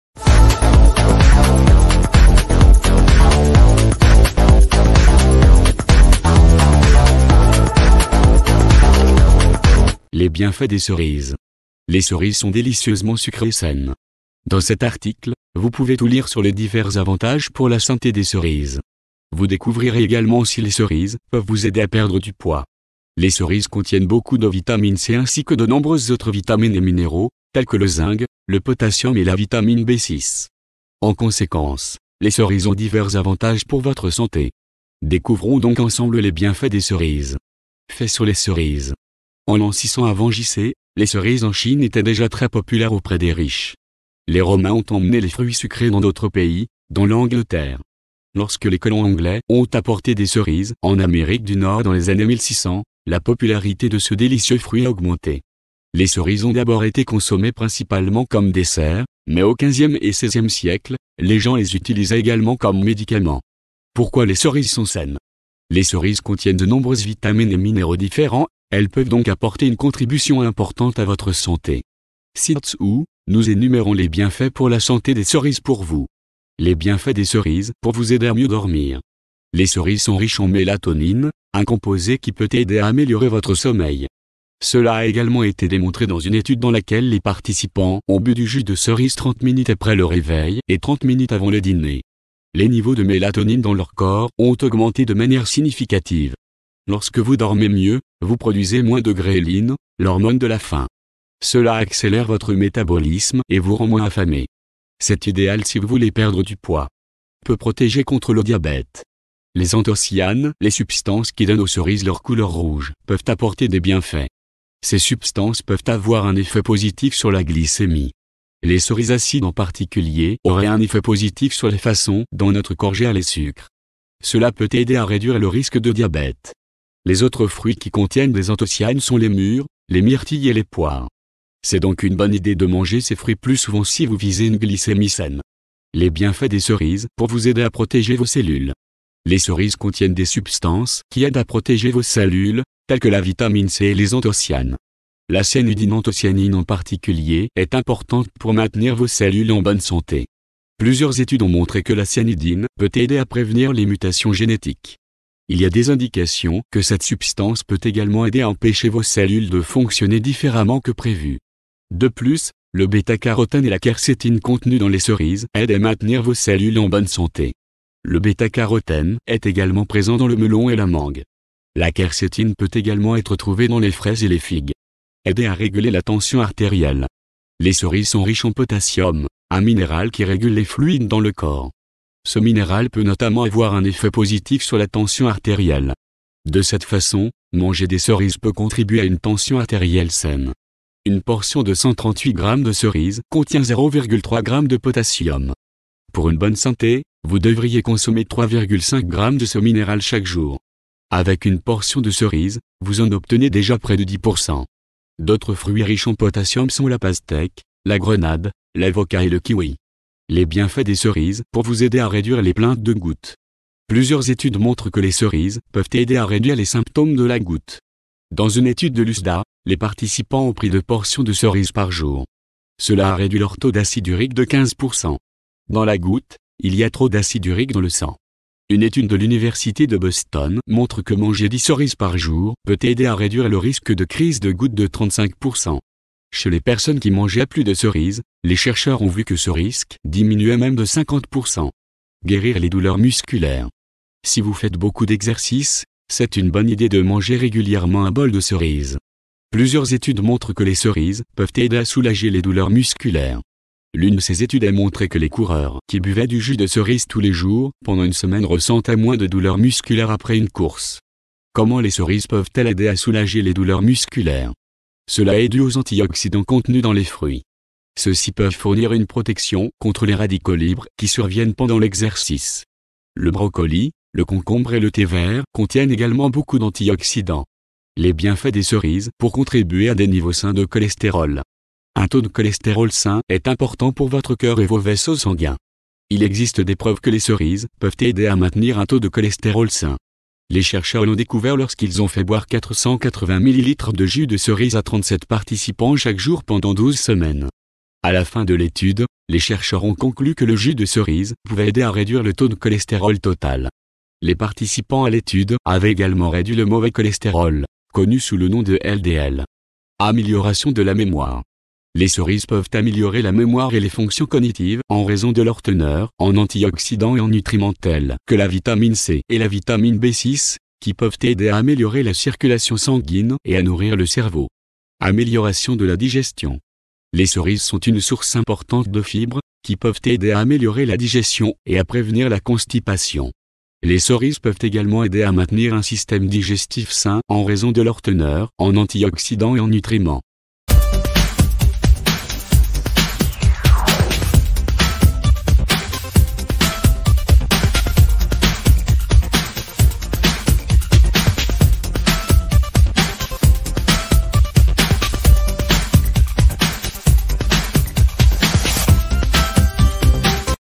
Les bienfaits des cerises
Les cerises sont délicieusement sucrées et saines. Dans cet article, vous pouvez tout lire sur les divers avantages pour la santé des cerises. Vous découvrirez également si les cerises peuvent vous aider à perdre du poids. Les cerises contiennent beaucoup de vitamine C ainsi que de nombreuses autres vitamines et minéraux, tels que le zinc, le potassium et la vitamine B6. En conséquence, les cerises ont divers avantages pour votre santé.